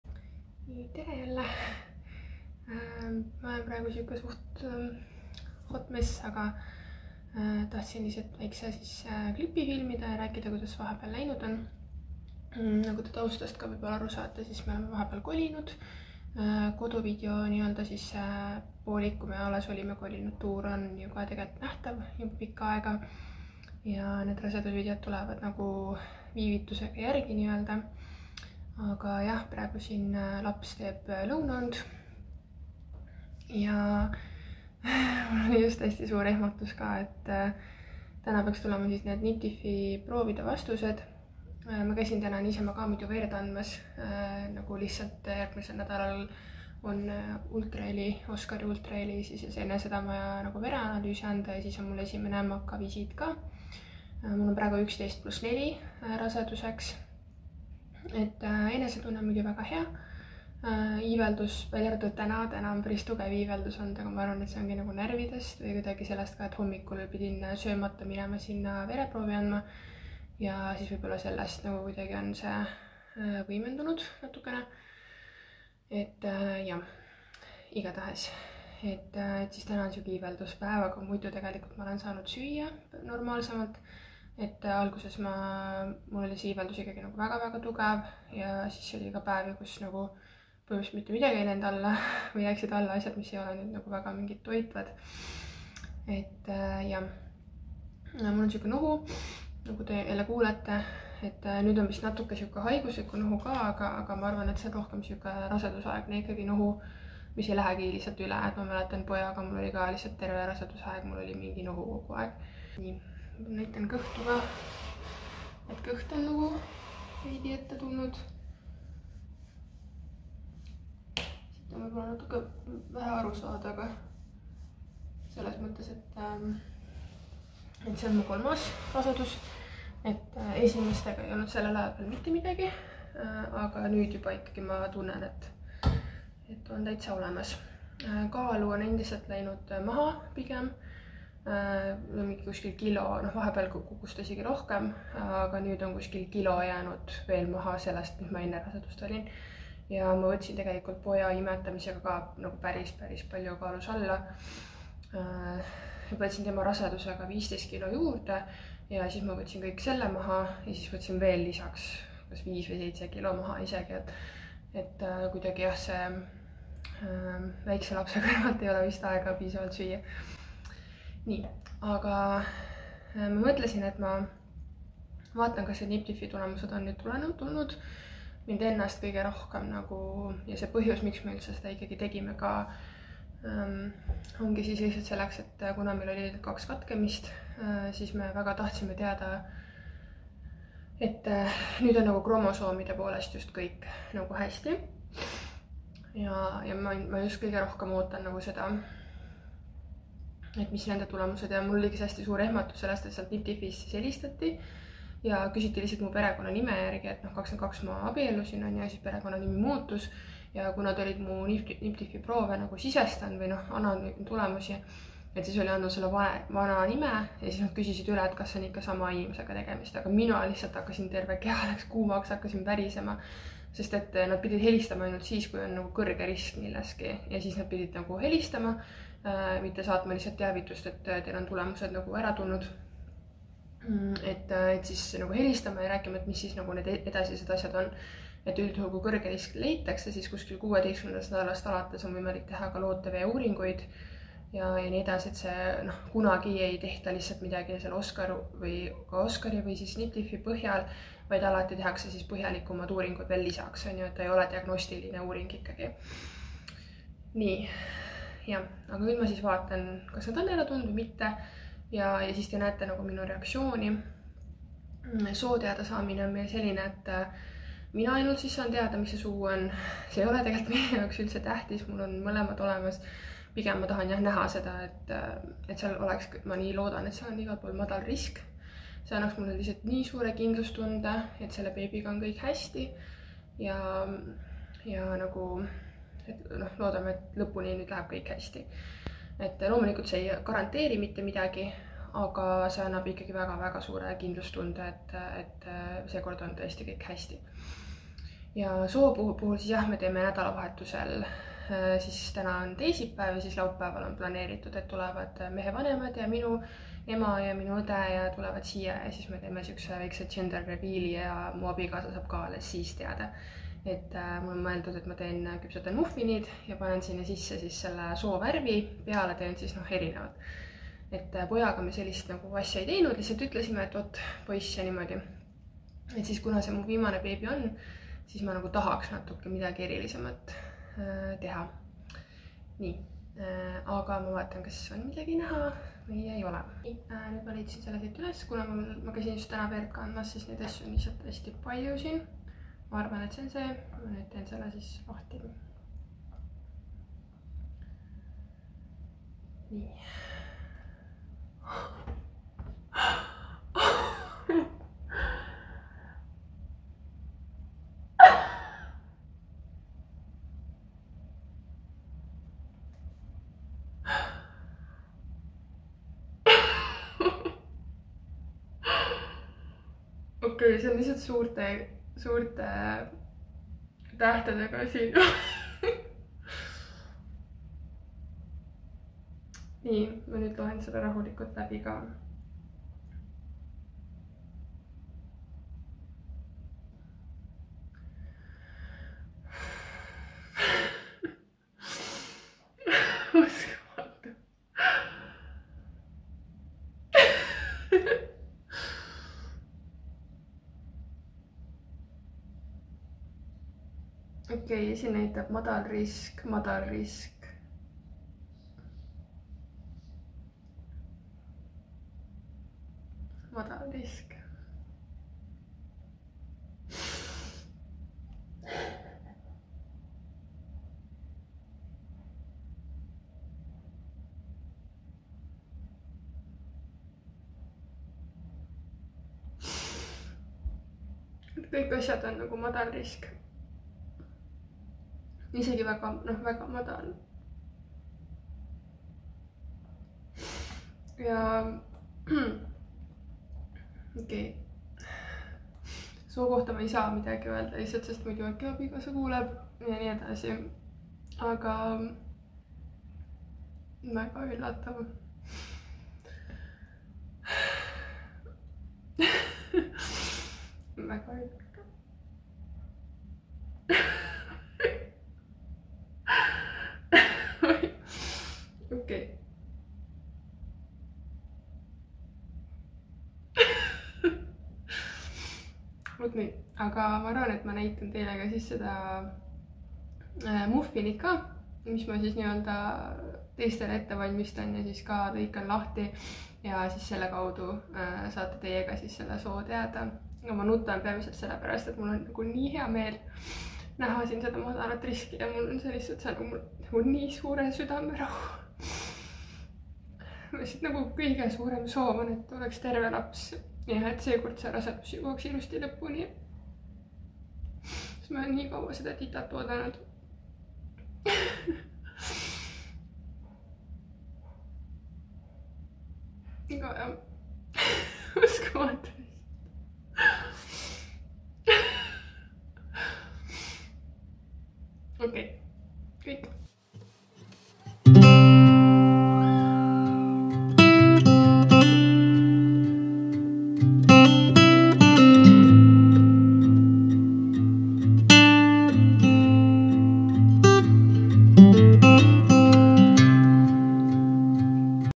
POISS VÕI TÜDRUK? Niptify testi tulemused? Pregnancy update and gender reveal
Selles videos avaldan Teile meie pesamuna soo ning räägin niptify testi tulemustest.

Tänan vaatamast!
Like videot ja Telli kanalit 💙🙏.

instagramis leiad meid @karupoegadepesa